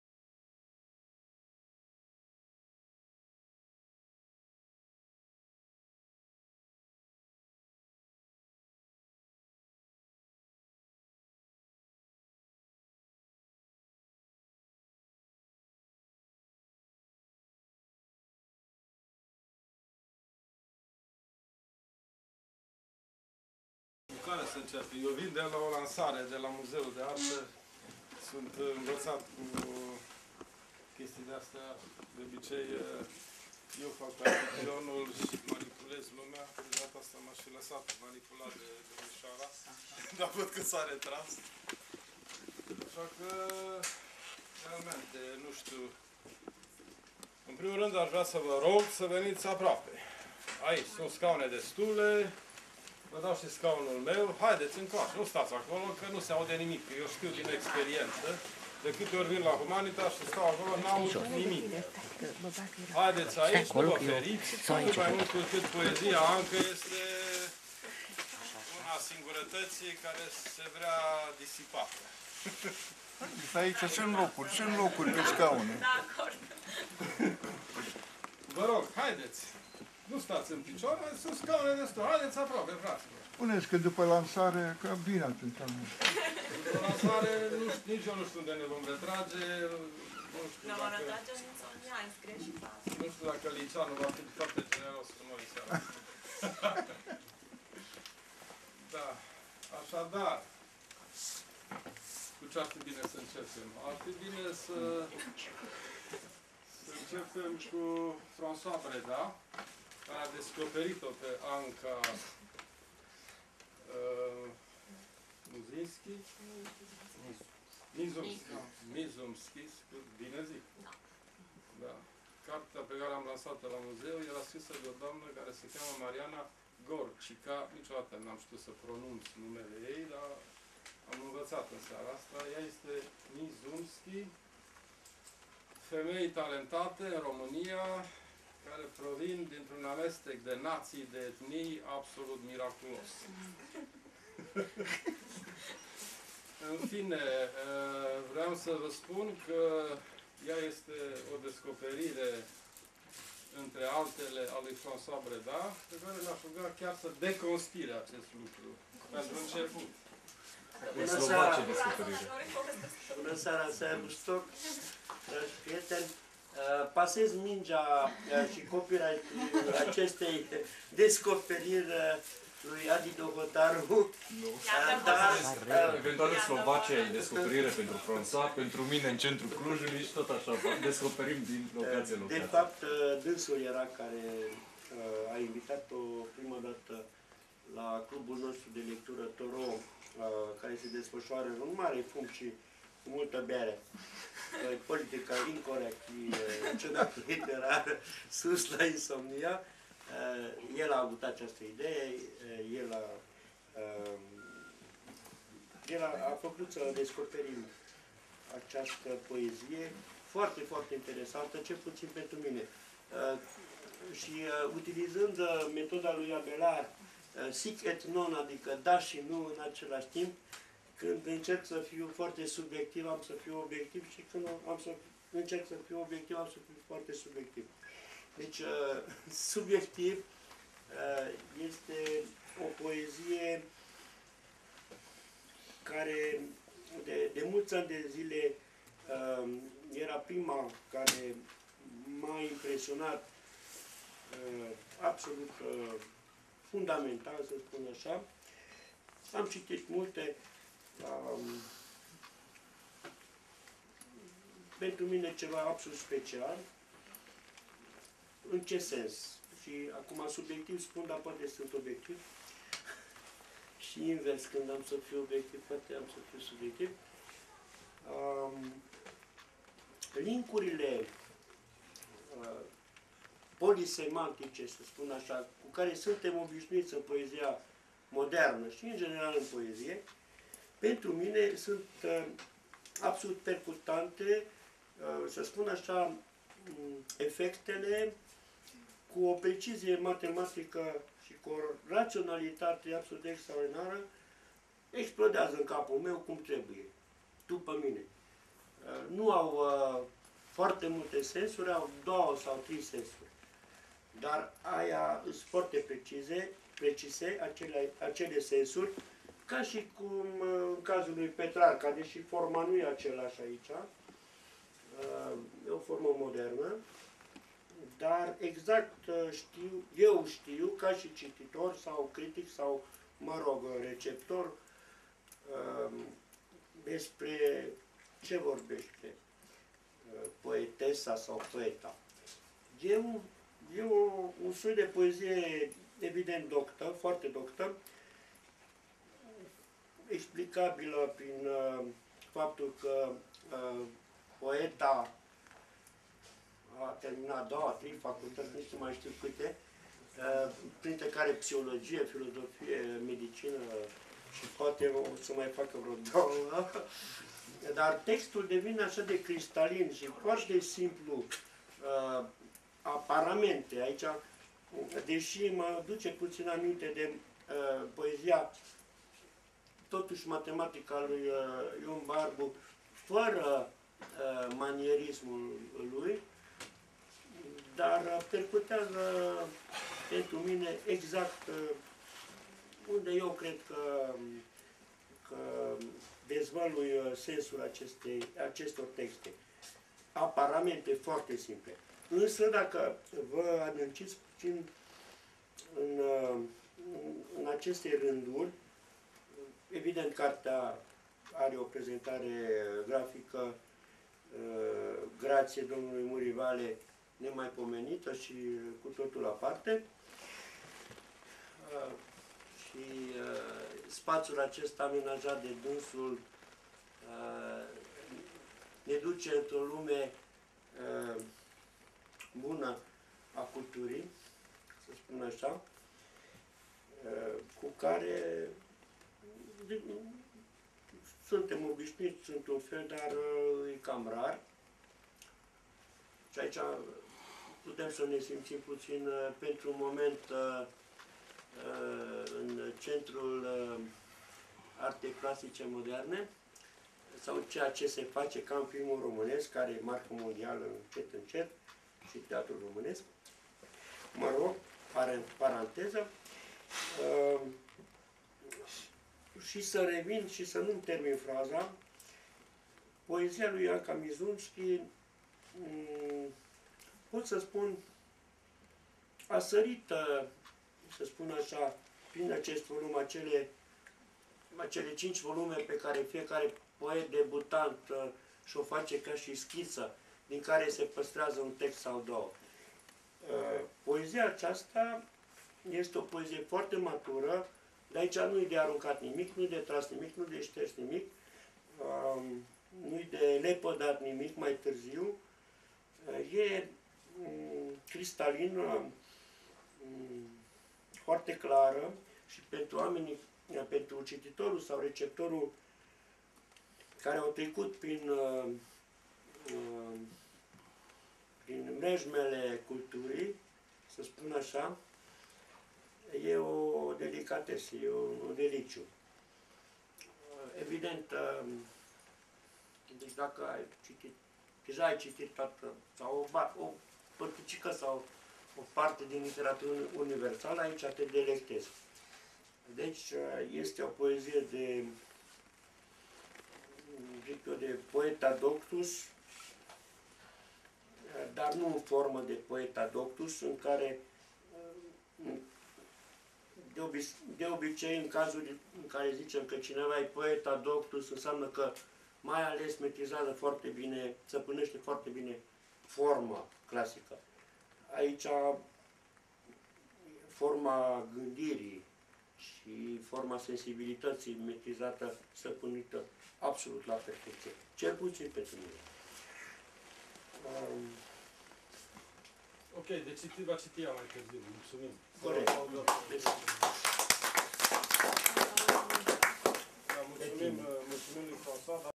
ANCA LUI NOE - ANCA MIZUMSCHI - SIXTINE ZERO - MURIVALE - FERENC BREDA -HUMANITAS- CLUJ.mpg
1 AM STRABĂTUT PODISUL TRANSILVAN SI INTR-UN TARZIU AM AJUNS LA CLUJ UNDE AM POPOSIT LA MAESTRUL CORNELIU BRUDASCU . IN ZIUA URMATOARE AM SUSPENDAT LUCRARILE MELE IN LIBRARIA HUMANITAS DIN CLUJ . DUPA AMIAZĂ LA ORA 17 00 H A FOST LANSAREA DE CARTE LA CARE AU PARTICIPAT . STEFAN MANASE , MIRCEA PETEAN , FRANCOIS BRENAC , CORNELIU BRUDASCU , CORNEL VANA , ADRIAN GRECU , SILVIA PETHEO ,ADELA TEODORU CRISTESCU O CALAUZĂ MINUNATĂ IN CLUJ  , SI INCA 20 DE PERSOANE . SEARA SA INCHEIAT LA CAFENEAUA LITERARA INSOMNIA . DIMINEATA AM STRANS LUCRARILE SI AM PLECAT SPRE BUCURESTI